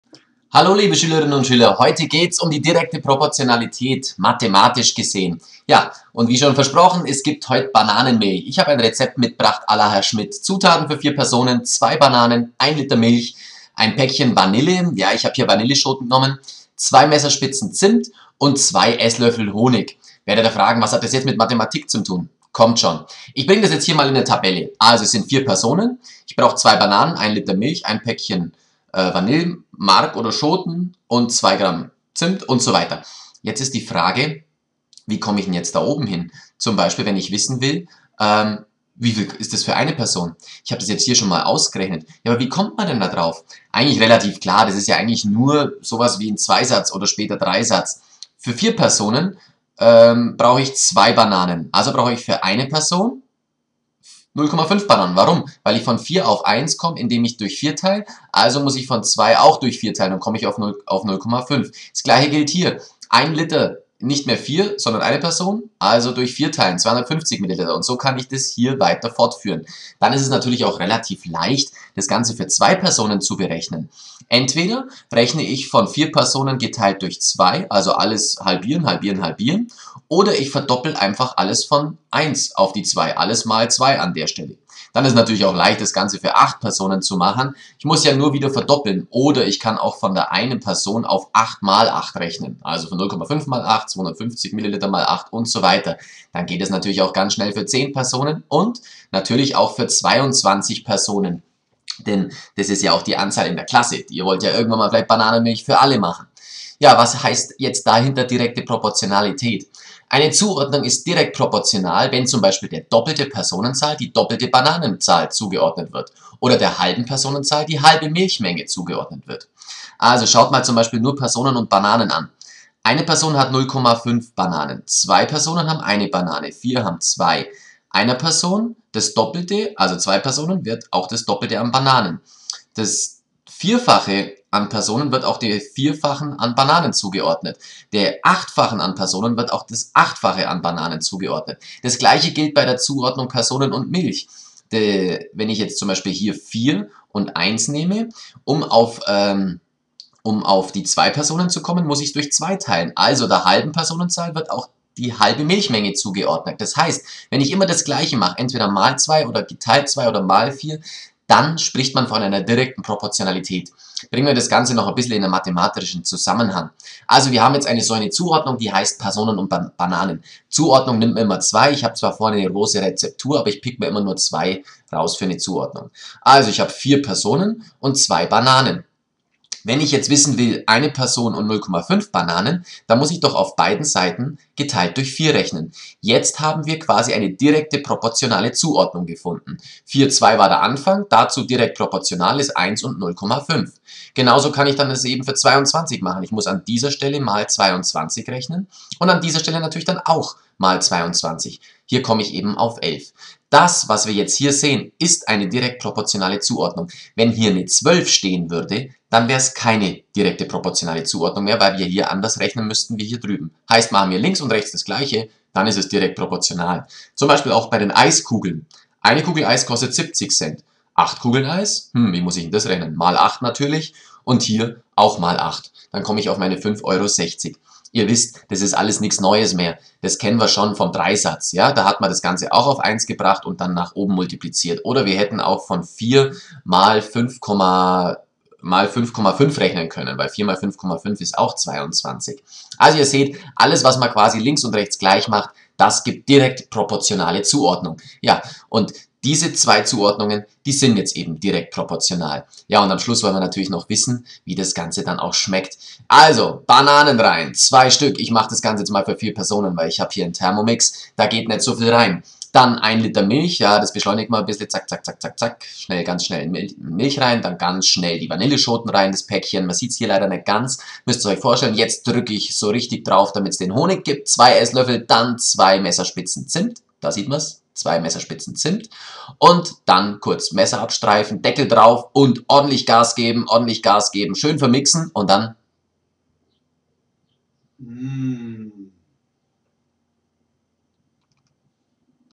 6.1 Direkte Proportionalität - mathematisch, bananisch
Achtung. in diesem Video ist ein Fehler: Beim Rezept für 10 Personen müssen die letzten beiden Werte 5 heißen und nicht 8.
Ansonsten zeige ich euch in diesem Video, was die Bananenmilch a la Herr Schmidt mit der direkten Proportionalität zu tun hat.